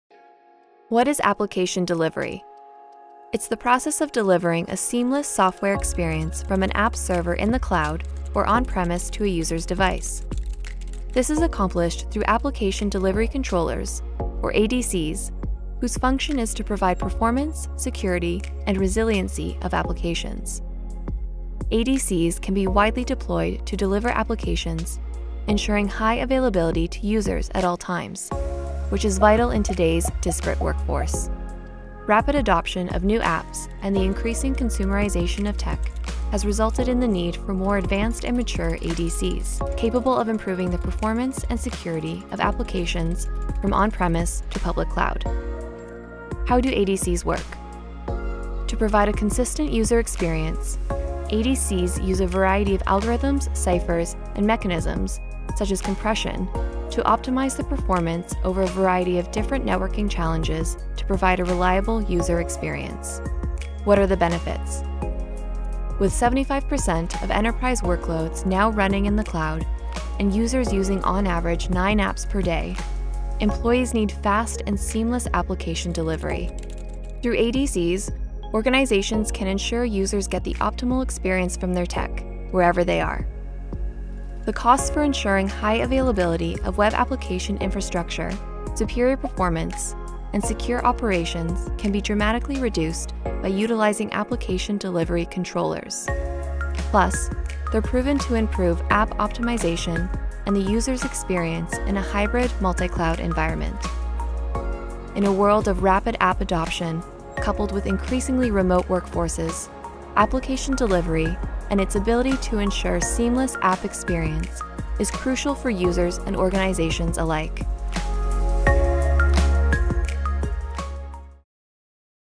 What is application delivery?
Rapid adoption of new apps and the increasing consumerization of tech has resulted in the need for more advanced and mature ADCs, capable of improving the performance and security of applications from on premise to public cloud.